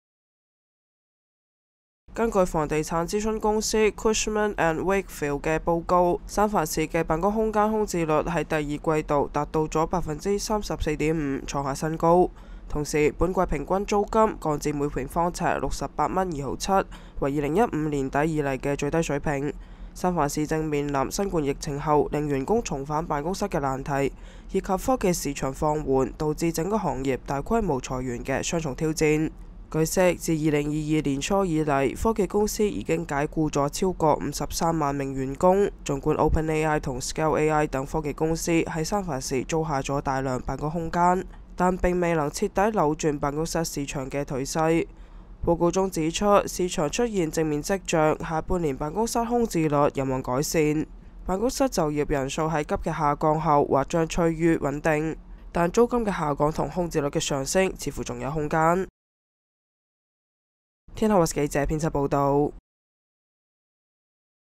【天下新聞】北加州: 三藩市辦公室空置率創新高 NorCal: San Francisco's office vacancy rate hits a new high
根據房地產諮詢公司Cushman&Wakefield的報告，三藩市的辦公空間空置率在第二季度達到了34.5%，創下新高。同時，本季平均租金降至每平方呎68.27元，為2015年底以來的最低水平。
三藩市正面臨新冠疫情後讓員工重返辦公室的難題以及科技市場放緩導致整個行業大規模裁員的雙重挑戰。據悉，自2022年初以來，科技公司已解雇了超過53萬名員工，
儘管Open AI和Scale AI等科技公司在三藩市租下了大量辦公空間，但並未能徹底扭轉辦公室市場的頹勢。
報告中指出，市場出現正面跡象，下半年辦公室空置率有望改善，辦公室就業人數在急劇下降後或將趨於穩定。但租金的下降和空置率的上升似乎還有空間。